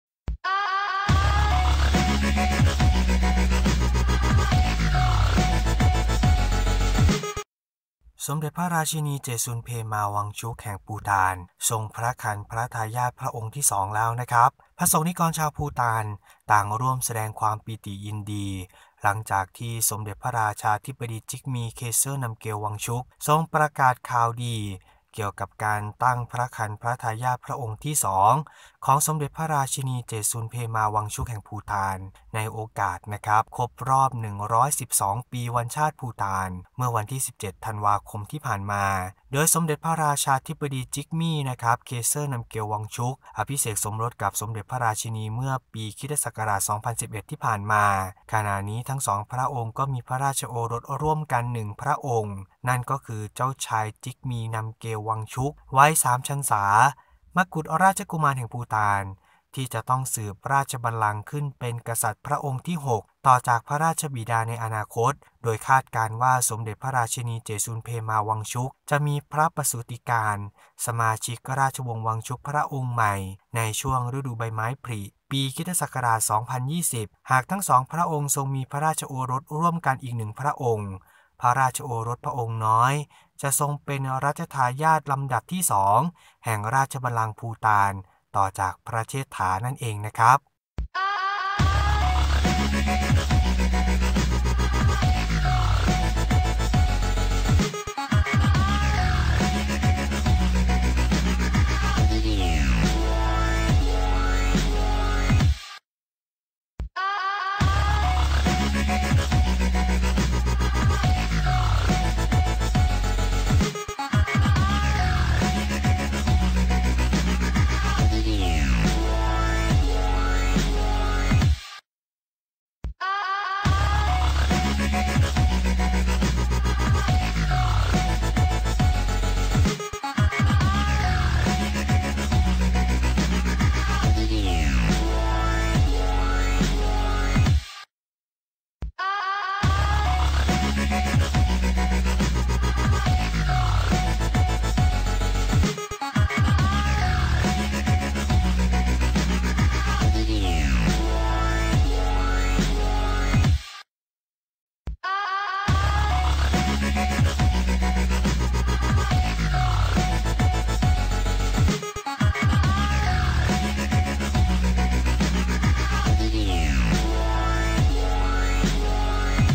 สมเด็จพระราชินีเจตซุน เพมา วังชุก แห่งภูฏาน ทรงพระครรภ์พระทายาทพระองค์ที่2 (เกาะกระแสเรื่องดังNo133)
สมเด็จพระราชินีเจตซุน เพมา วังชุก แห่งภูฏาน ทรงพระครรภ์พระทายาทพระองค์ที่ 2 (เกาะกระแสเรื่องดัง No.133)
พสกนิกรชาวภูฏานต่างร่วมแสดงความปีติยินดี หลังจากสมเด็จพระราชาธิบดีจิกมี เคเซอร์ นัมเกล วังชุก ทรงประกาศข่าวดีเกี่ยวกับการตั้งพระครรภ์พระทายาทพระองค์ที่ 2 ของสมเด็จพระราชินีเจตซุน เพมา วังชุก แห่งภูฏาน ในโอกาสครบรอบ 112 ปี วันชาติภูฏาน เมื่อวันที่ 17 ธันวาคมที่ผ่านมา

โดยสมเด็จพระราชาธิบดีจิกมี เคเซอร์ นัมเกล วังชุก อภิเษกสมรสกับสมเด็จพระราชินีเมื่อปี 2011 ที่ผ่านมา ขณะนี้ทั้งสองพระองค์มีพระราชโอรสร่วมกัน 1 พระองค์คือ เจ้าชายจิกมี นัมเกล วังชุก วัย 3 ชันษา มกุฎราชกุมารแห่งภูฏานที่จะต้องสืบราชบัลลังก์ขึ้นเป็นกษัตริย์พระองค์ที่ 6 ต่อจากพระราชบิดาในอนาคต

คาดการณ์ว่าสมเด็จพระราชินีเจตซุน เพมา วังชุก จะมีพระประสูติกาลสมาชิกราชวงศ์วังชุกพระองค์ใหม่ในช่วงฤดูใบไม้ผลิ ปี 2020 หากทั้งสองพระองค์ทรงมีพระราชโอรสร่วมกันอีก 1 พระองค์ พระราชโอรสพระองค์น้อยจะทรงเป็นรัชทายาทลำดับที่ 2 แห่งราชบัลลังก์ภูฏานต่อจากพระเชษฐา